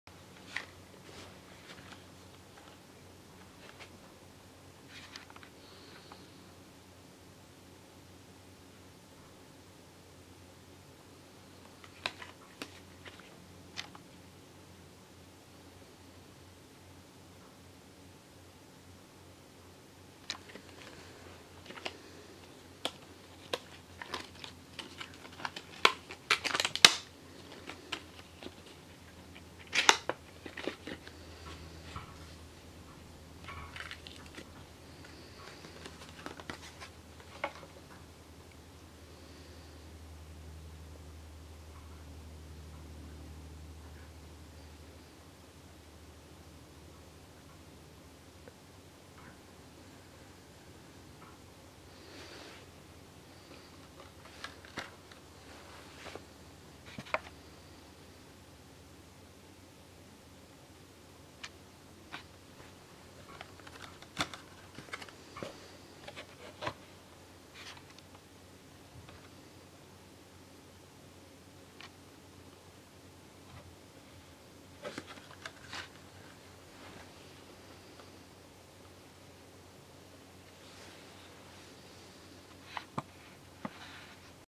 【開封動画】サンワサプライ 雷ガードタップ 3個口 開封 3 mouths unboxing Sanwa thunder guard tap
雷から接続している機器を守ることができるみたいです。